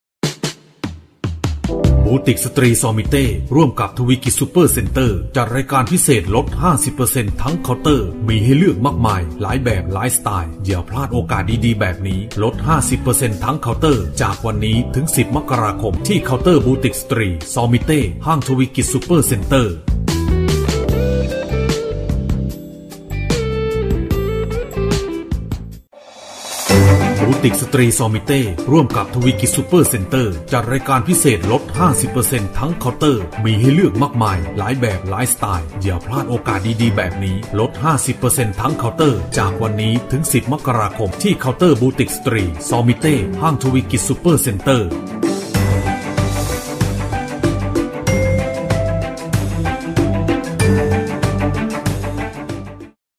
ห้างทวีกิจฯ l โฆษณาซอมิเต้ ลด 50%
โฆษณาซอมิเต้ ลด 50% ที่ห้างทวีกิจซุปเปอร์เซ็นเตอร์ จ.บุรีรัมย์